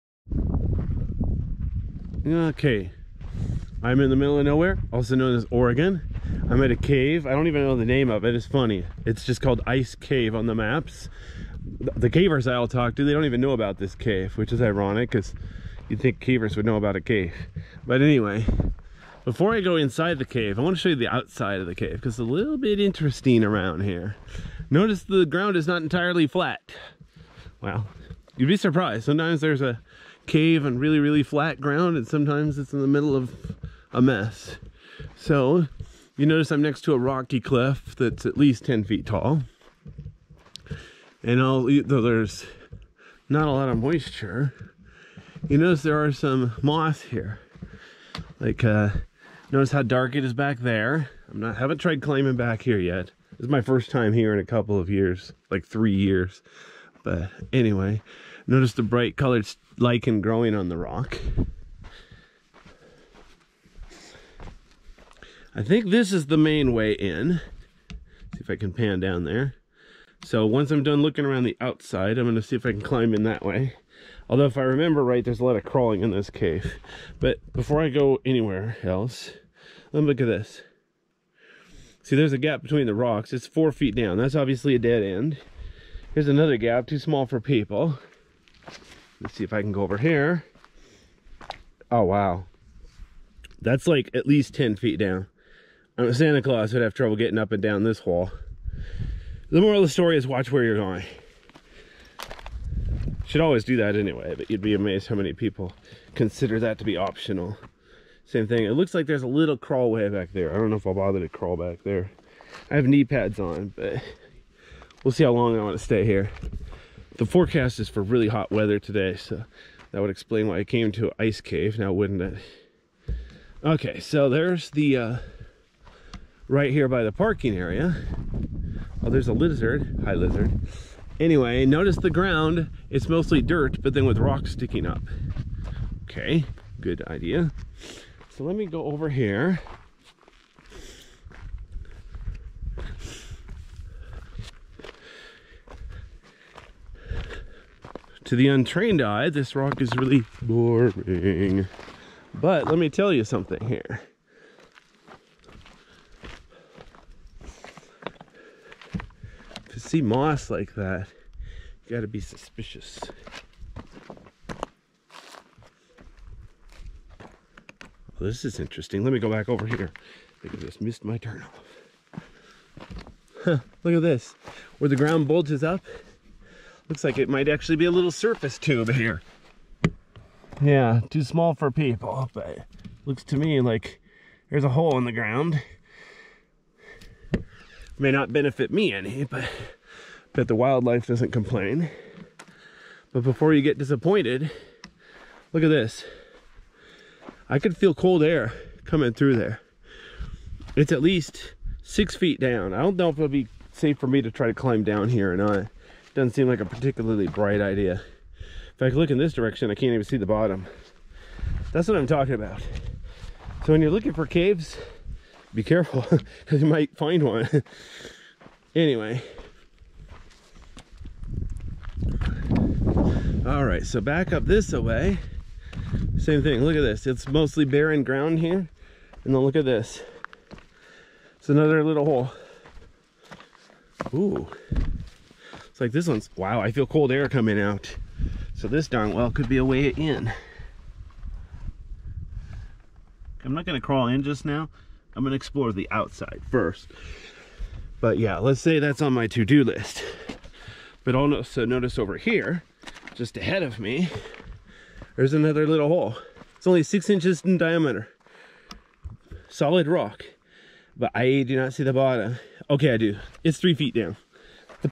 ice cave pothole, video 2, part 1, 5k 30fps 2880p30
This is Loma Vista Cave in central Oregon, but the maps call it ice cave pothole.